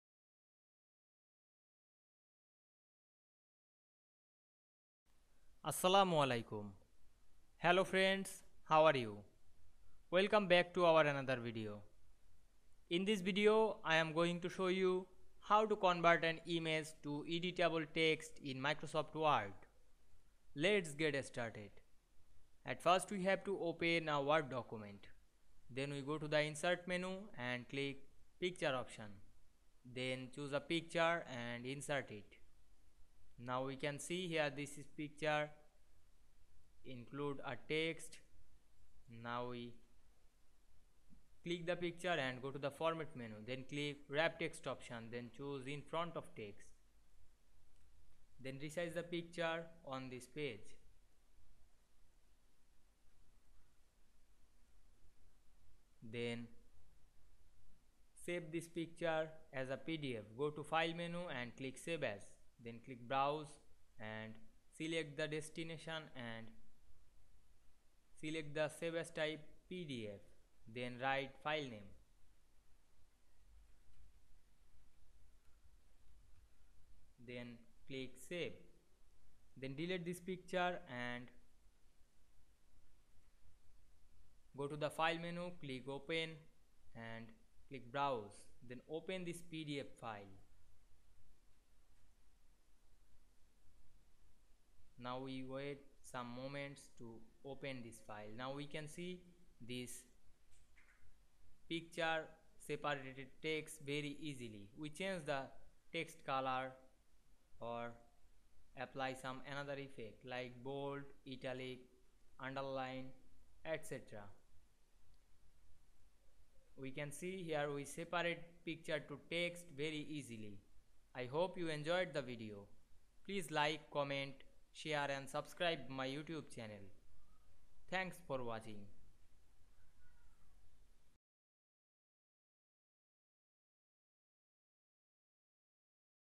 How to separate text from a picture in Word
Assalamu Walaikum,
In this video I will show you, How to separate text from a picture in Word. Let's get started.